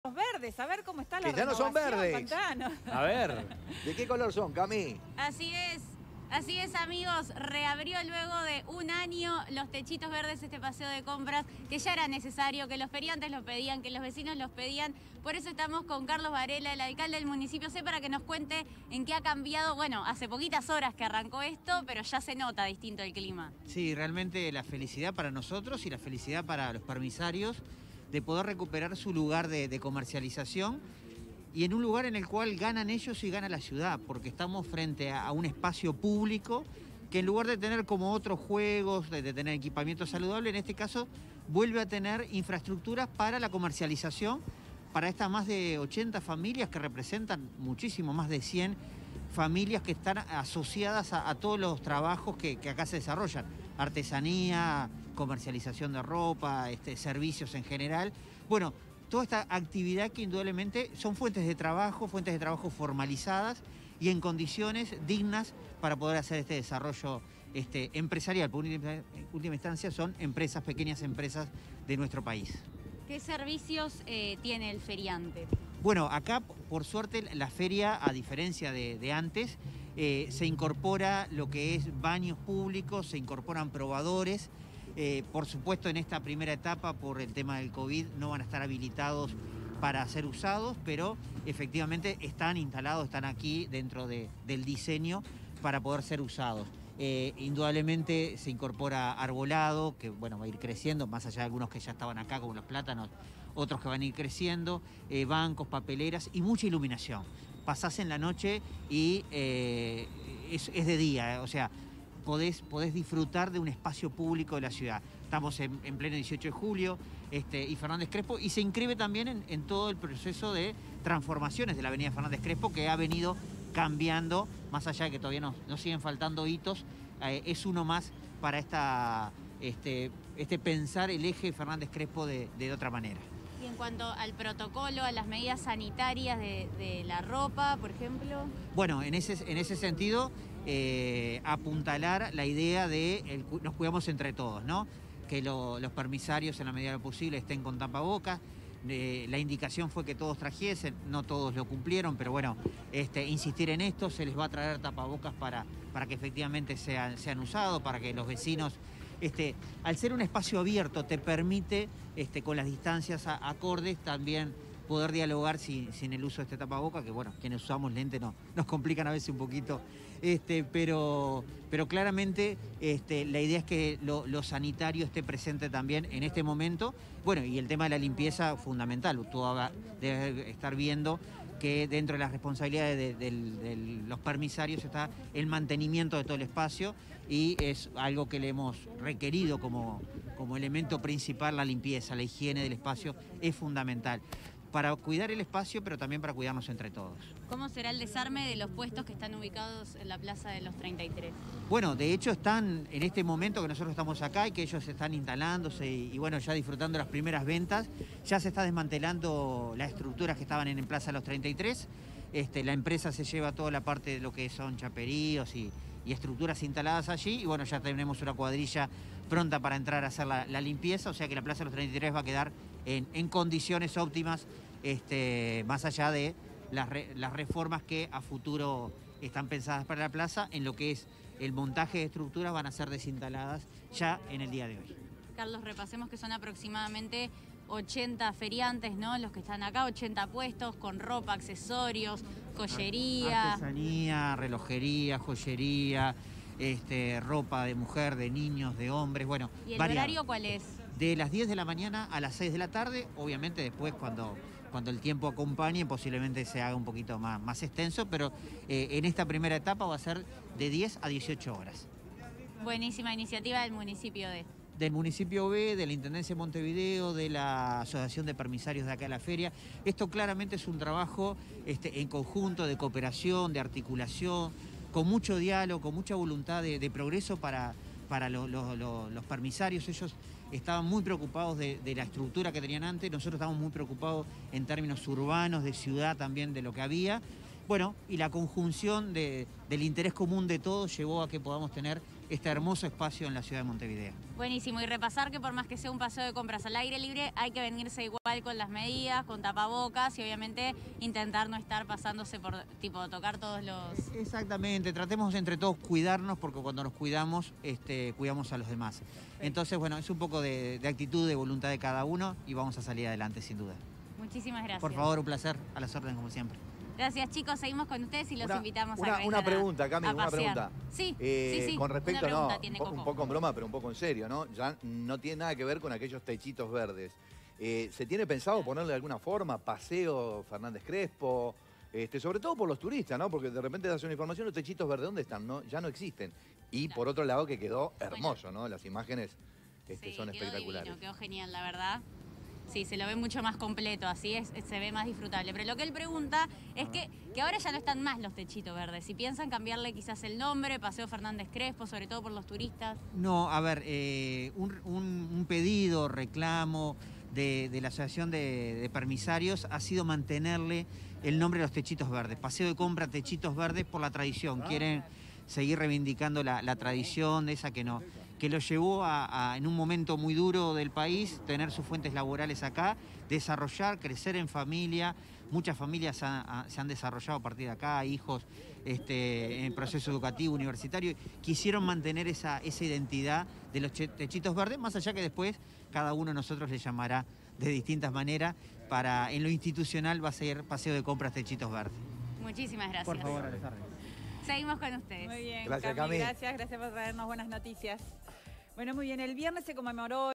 Volvieron los techitos verdes renovados
Con una obra y pandemia de por medio, los puestos de Fernández Crespo y Colonia volvieron a su lugar original.